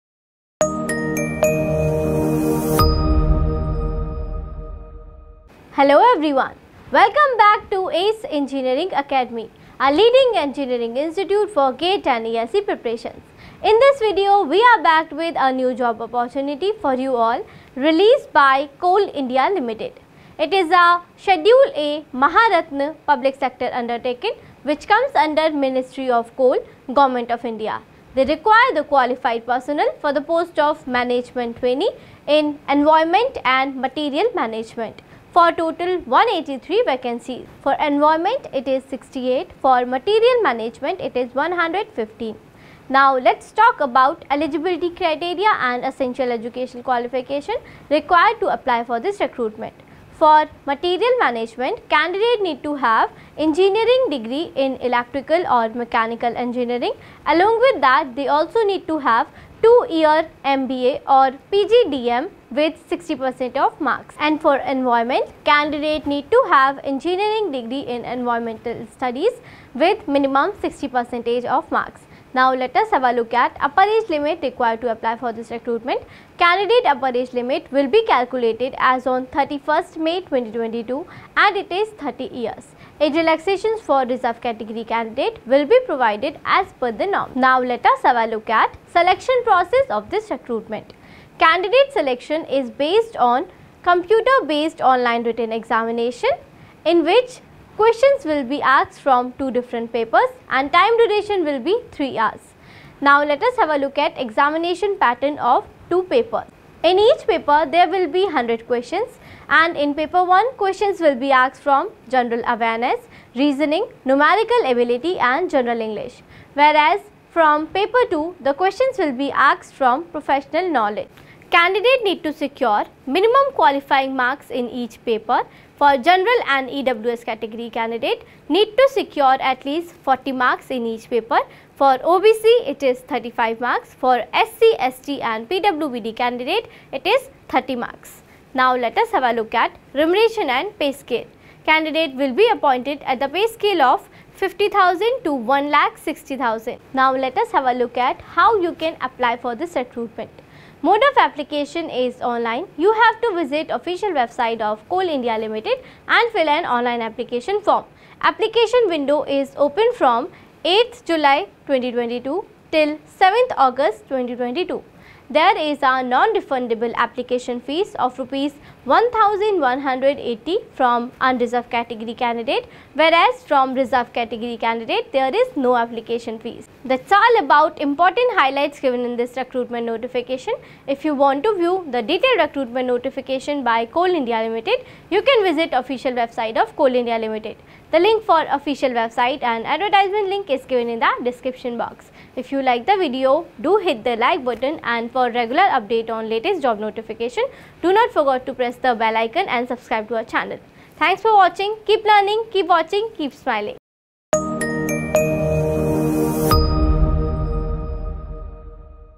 CIL invites application for the post of Management Trainee | Vacancies: 183 | Last Dt: 07.08.2022
POST NAME:
MANAGEMENT TRAINEE

DISCPLINE:
 Environment: 68
 Materials Management: 115

ELIGIBILTY CRETERIA:
 Materials Management: Engineering Degree in Electrical or Mechanical Engineering with 2 years full time MBA/ PG Diploma in Management with minimum 60% marks.

 Environment:
Degree in Environmental Engineering with minimum 60% marks Or any Engineering Degree with PG Degree/Diploma in Environmental Engineering from a recognized University/Institute with minimum 60% marks.

UPPER AGE LIMIT:
30 Years as on 31-May-2022

SELECTION PROCESS:
Computer Based Online Test
EXAMINATION PATTERN:
Duration: 3 Hours
Two papers (Paper-I & Paper-II) of 100 marks each.
100 MCQ Questions Each Paper.

PAPER-I :
 General Knowledge/Awareness
 Reasoning
 Numerical Ability
 General English

PAPER-II:
Professional Knowledge (Discipline related)

Qualifying marks in the Computer Based
Online Test:
 GENERAL (UR) / EWS:Minimum 40 marks in each
 OBC (Non-Creamy Layer):Minimum 35 marks in each
 SC / ST / PwD: Minimum 30 marks in each

PAY SCALE:
Rs. 50,000 -3%-1,60,000

Mode: Online
APPLICATION FEES:
 General/OBC(NCL)/EWS category: 1180/-
 SC/ST/PwBDs/Ex-Servicemen need not pay the Registration fee

IMPORTANT DATE:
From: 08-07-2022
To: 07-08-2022